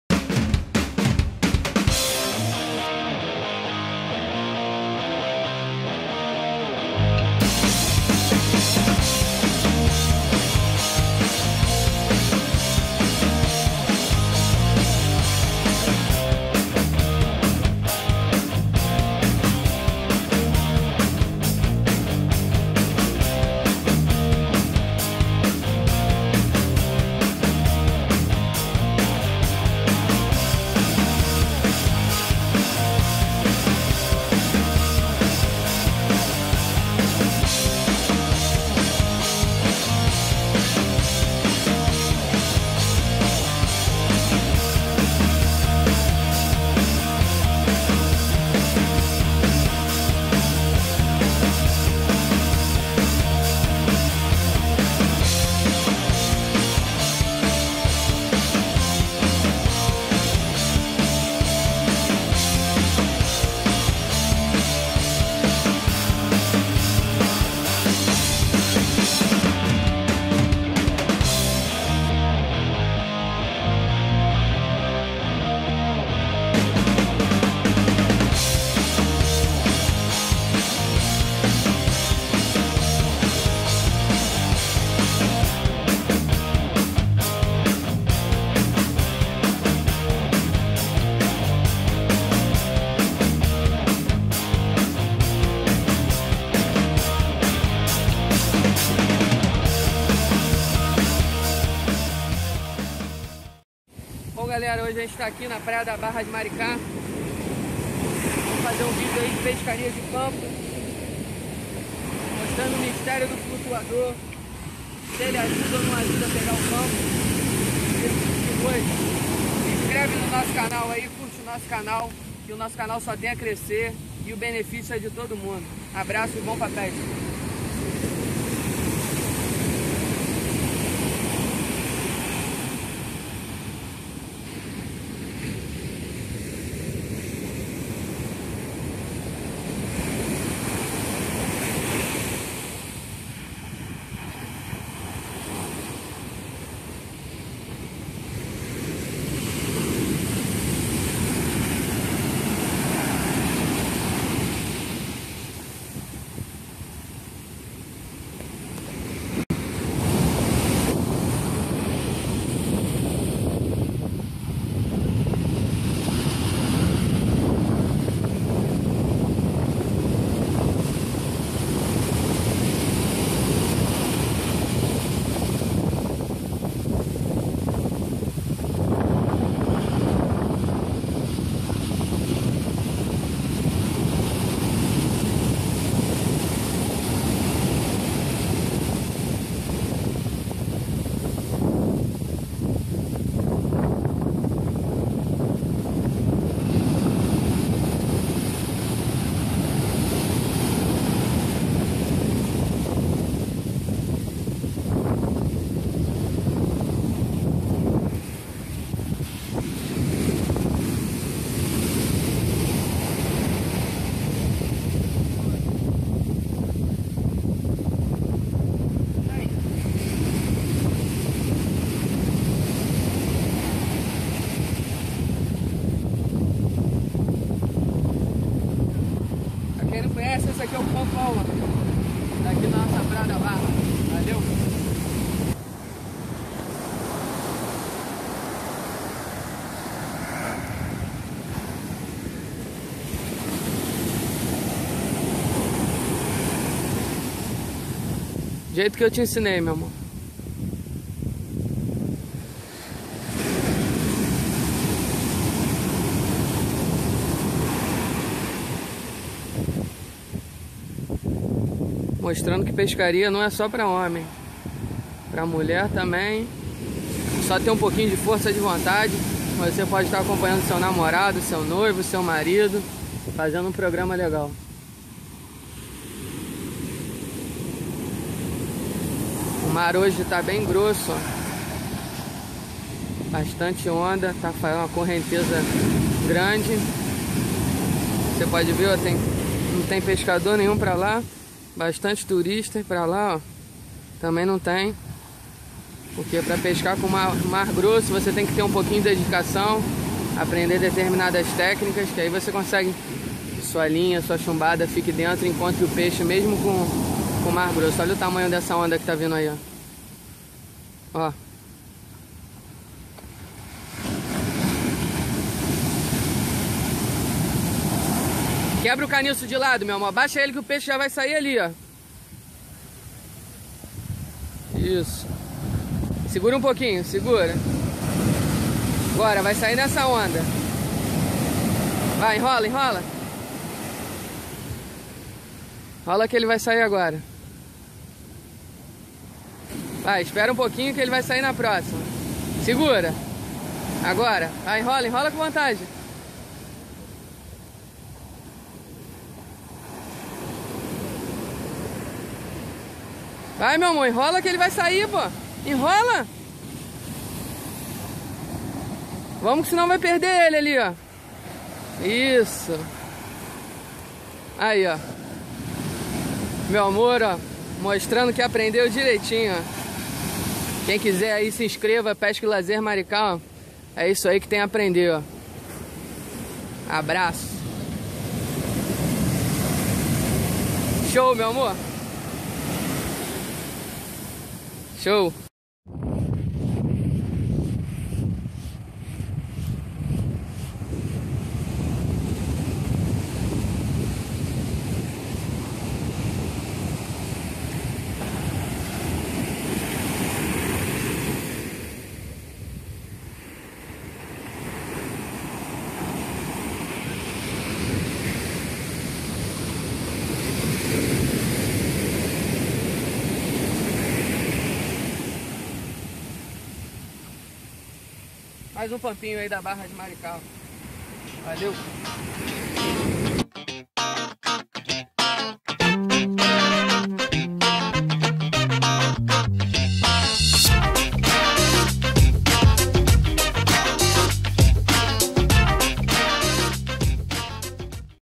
Pesca de Praia Pampo no flutuador(boinha) Long Cast
6° EPISÓDIO do canal Pesca e Lazer Maricá, mostrando a eficiência do flutuador em algumas condições para facilitar a sua pescaria de pampos, este vídeo vai revolucionar a sua pescaria de maneira simples,  4 anos atrás comecei a pescar com auxilio do flutuador depois de ver um vídeo de um amigo da Flórida-EUA e hoje estou compartilhando com vocês este conhecimento, abraço e sucesso em suas pescarias

# 7° EP Dicas de como confeccionar seu chicote e como utilizar o flutuador corretamente...

# SEMPRE TEMOS ALGO A APRENDER